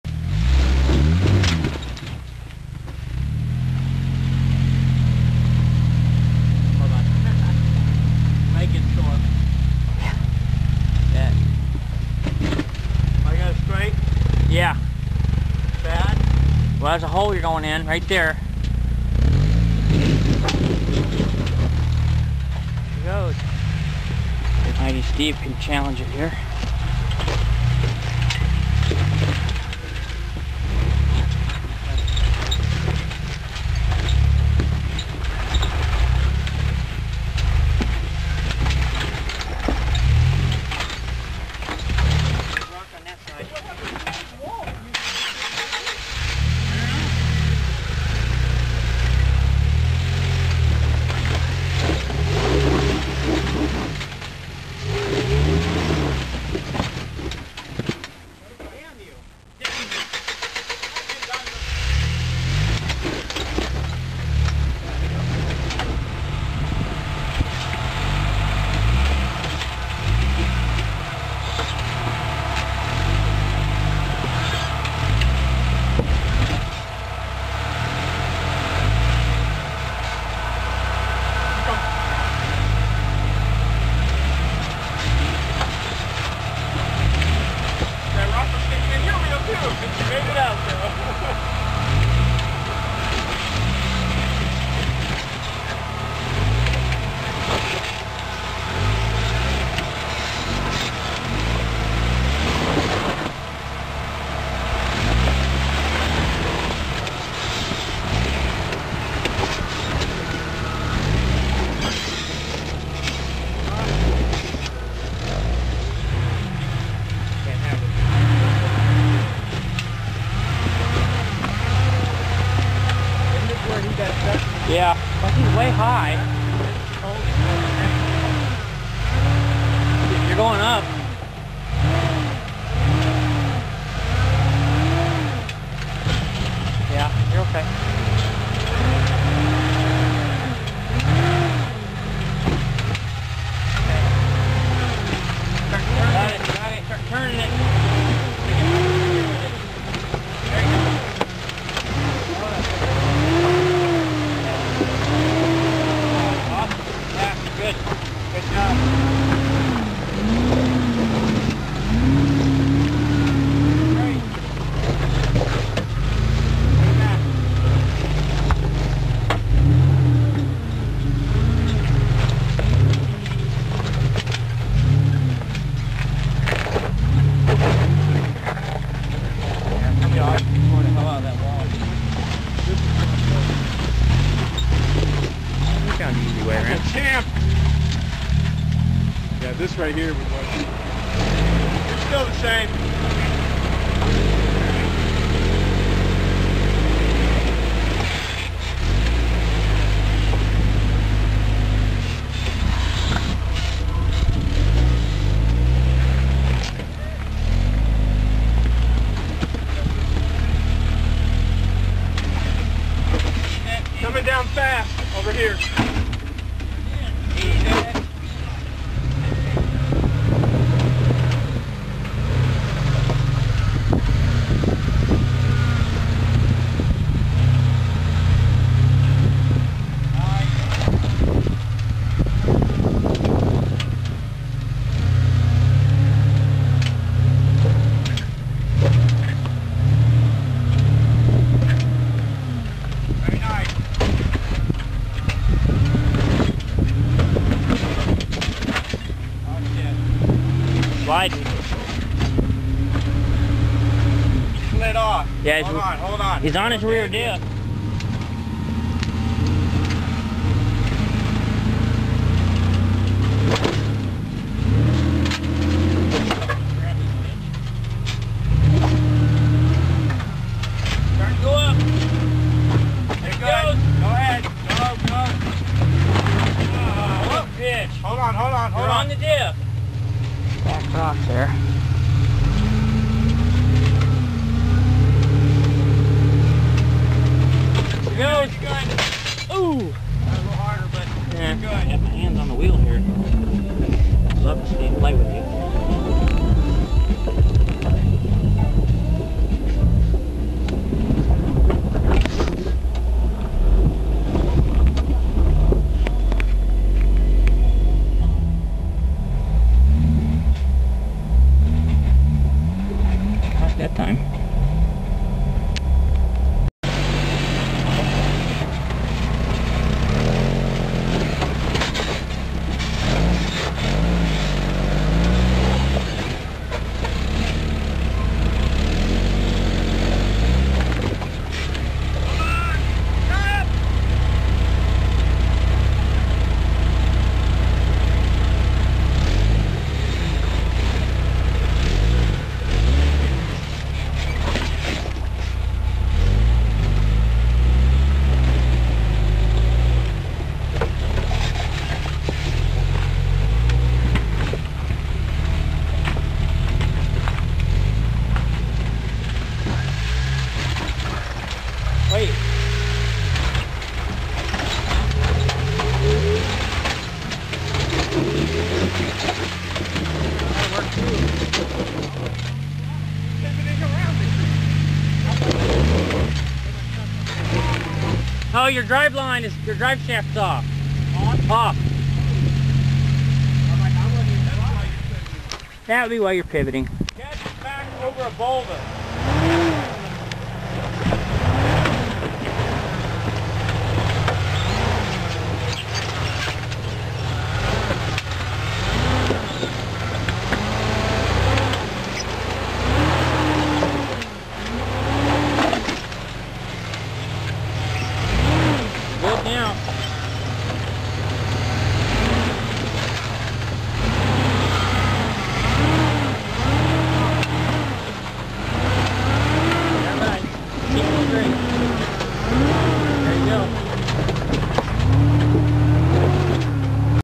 Firebird 4x4 arizona
A little harder of a rock crawling trail.